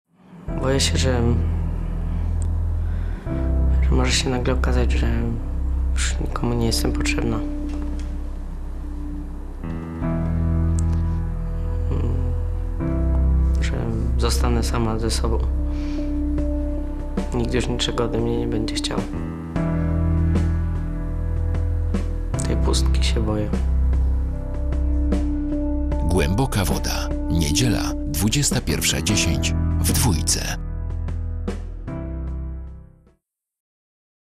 Głęboka woda - Agnieszka Podsiadlik. Czego się boi serialowa Małgosia?
Poprosiliśmy aktorów, aby wcielając się w swoje serialowe postacie odpowiedzieli nam na kilka pytań: Czego się boisz? Co cię wzrusza? O czym marzysz? Oto ich odpowiedzi.

Reszta filmów na kanale mediabrigadepl